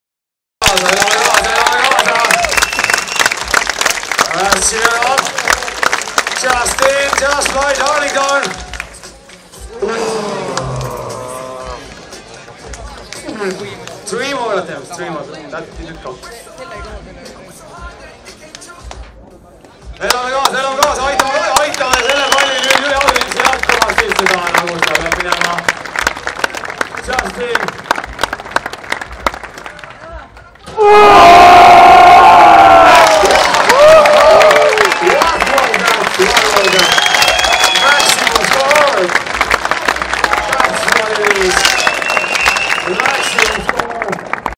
Justin "Jusfly" Darlington @ Tallinn Open, Estonia
Dunk contest Tallinn Open fiba 3on3